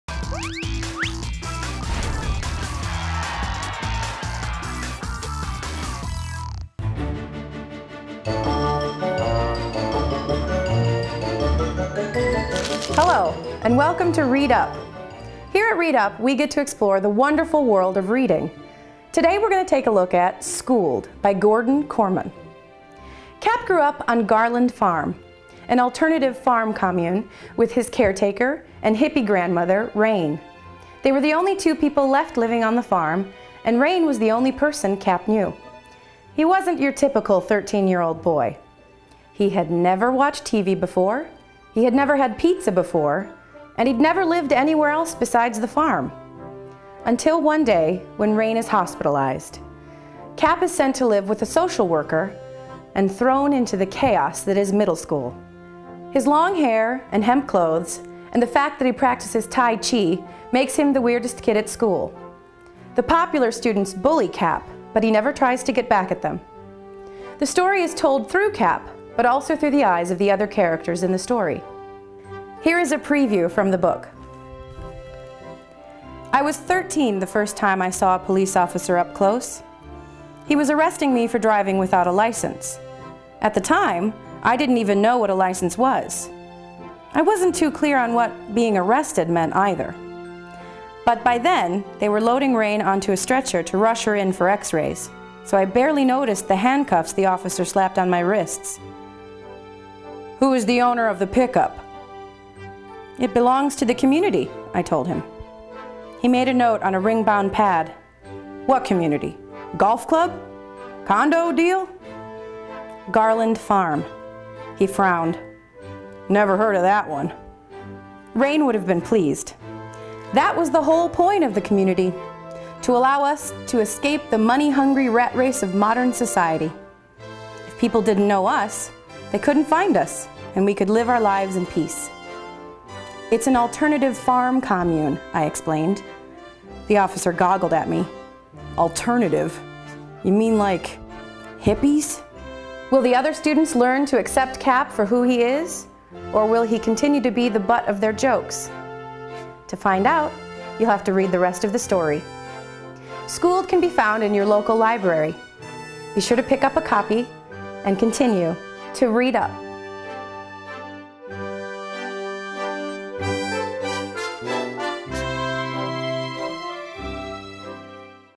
LRN 101 - Read Up: Schooled
Keep Learning Productions presents LRN 101 Episode 11: Read Up

In this episode, you'll learn about the book Schooled by Gordan Korman